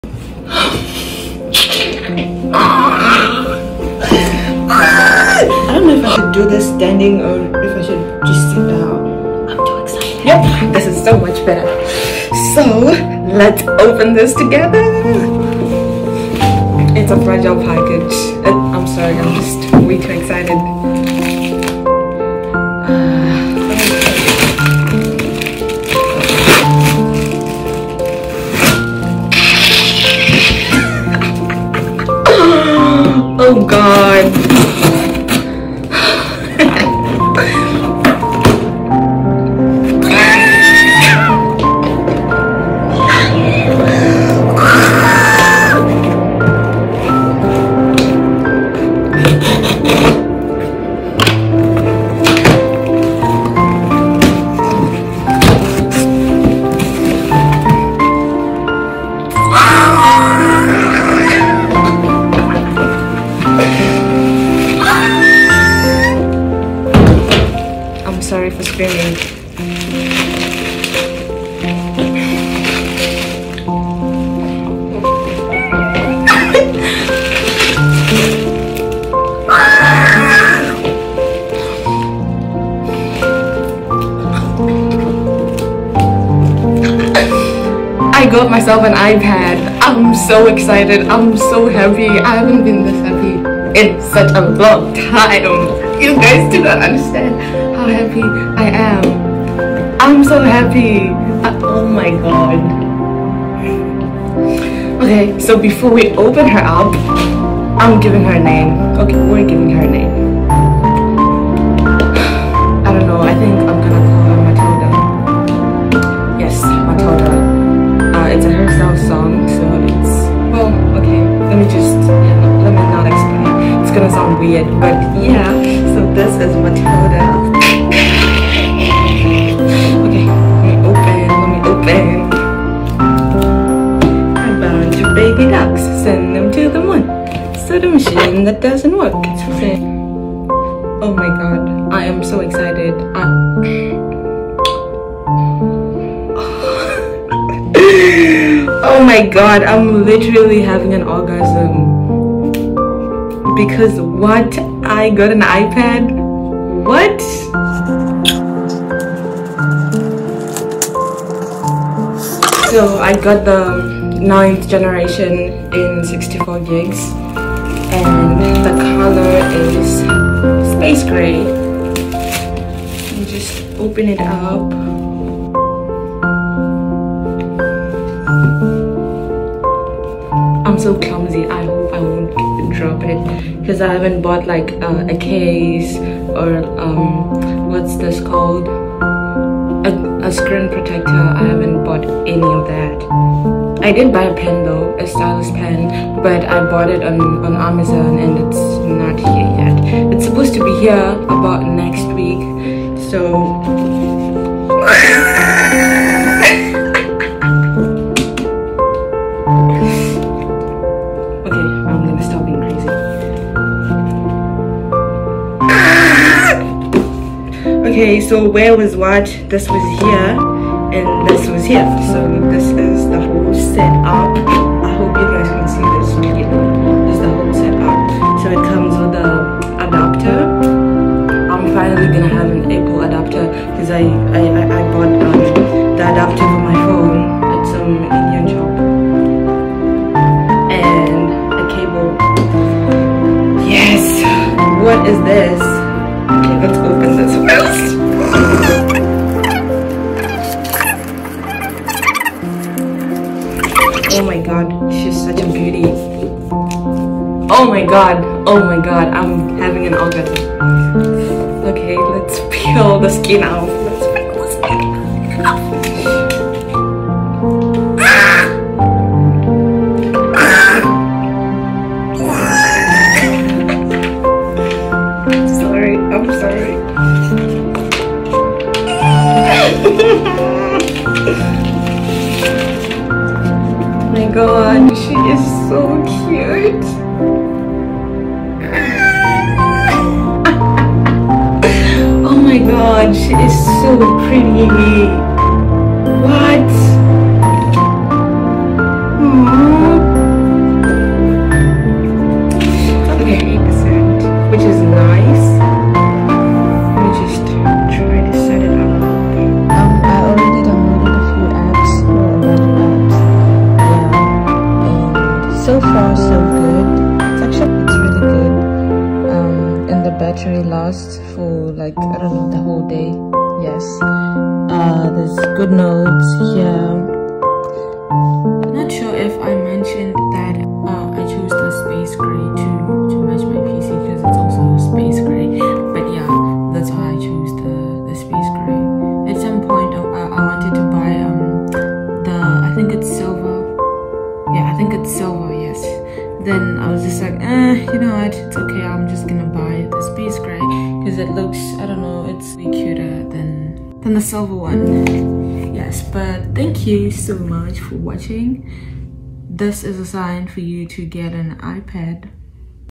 iPad 9th gen unboxing 💕
I got myself the ipad 9th gen for my 20th birthday 😅.Please note that i originally took this for TikTok which is why it’s not on landscape or YouTube standard ,but nevertheless I hope you enjoy watching this 💌